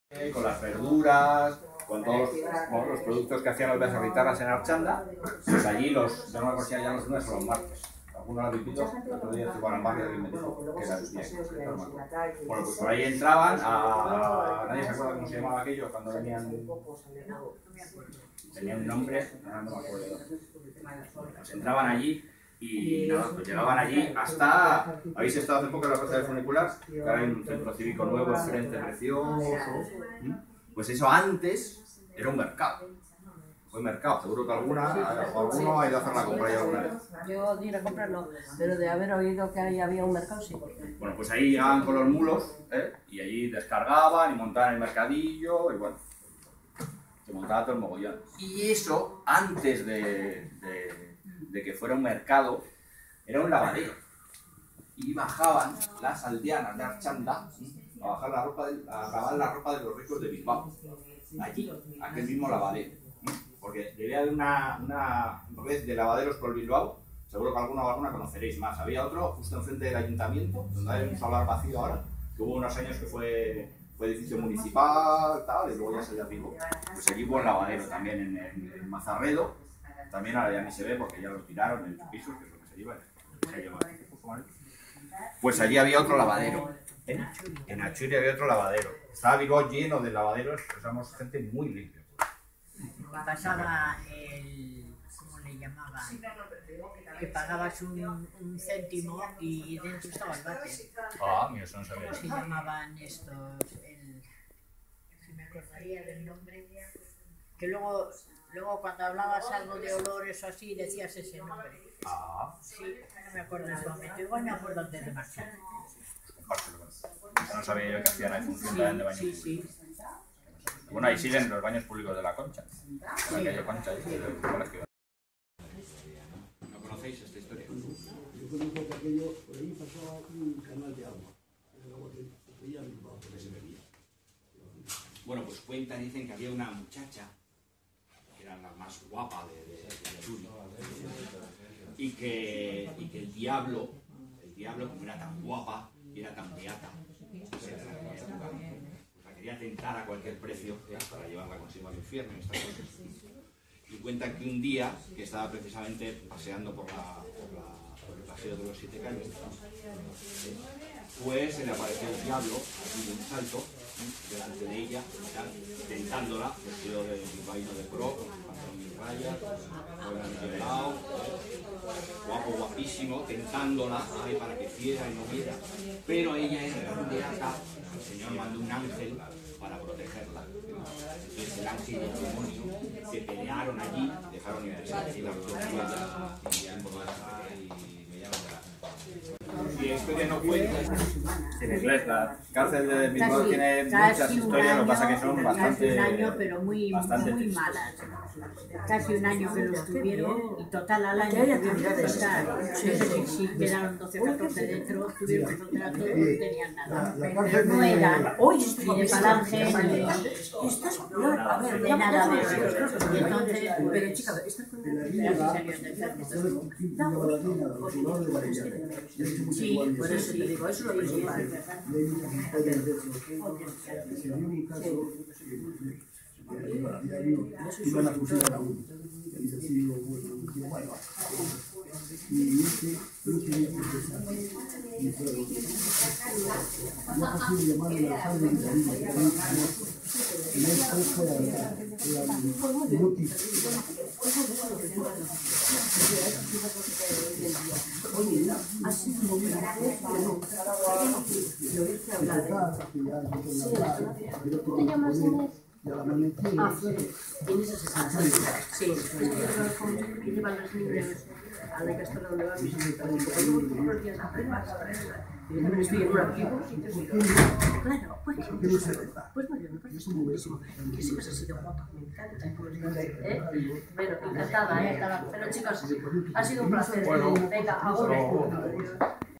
10º Aniversario Abuelo Actual: Taller cuenta cuentos en el mes de Abril
Taller de cuenta cuentos a cargo de Pedro Ruiz de Alabazan